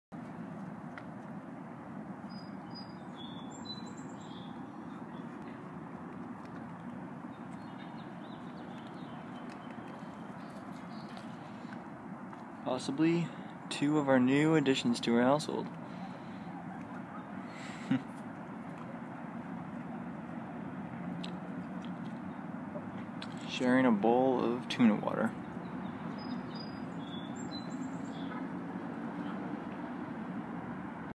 Super Cute, Tiny Kittens Rescued with Tuna Juice!
We are Bunny and Bruiser! We were abandoned near the Robbins' apartment. We were extremely hungry and tuna juice was exactly what made us fall in love with our new parents!

Follow us, kittens Bunny & Bruiser, as we go from tiny babies to speedy kitties!